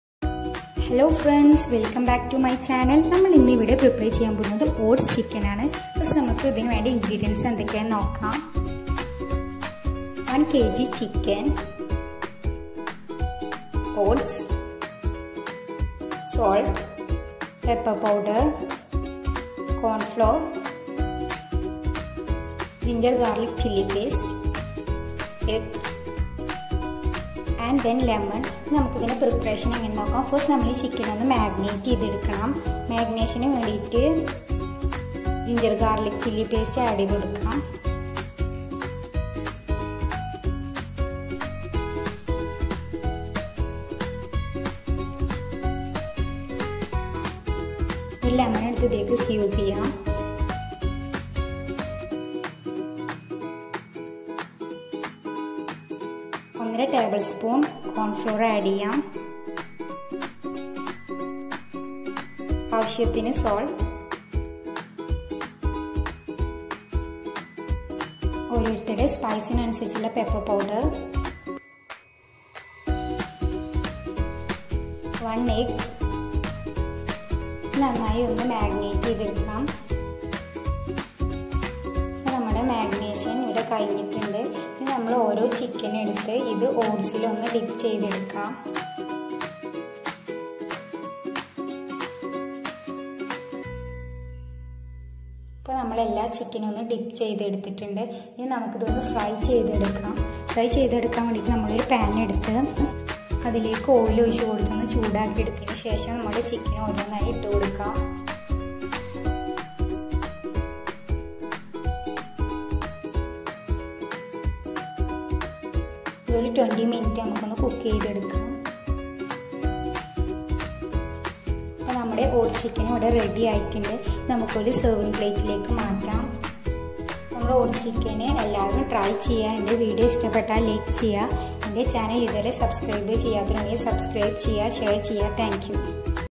||Chicken oats recipe||kids special KFC||
oats chicken#kids special recipe#
simple and yummy home made kfc##
Ingredients:
Chicken
Egg
Oats
Salt
Corn flour
Ginger garlic chilly paste
Lemon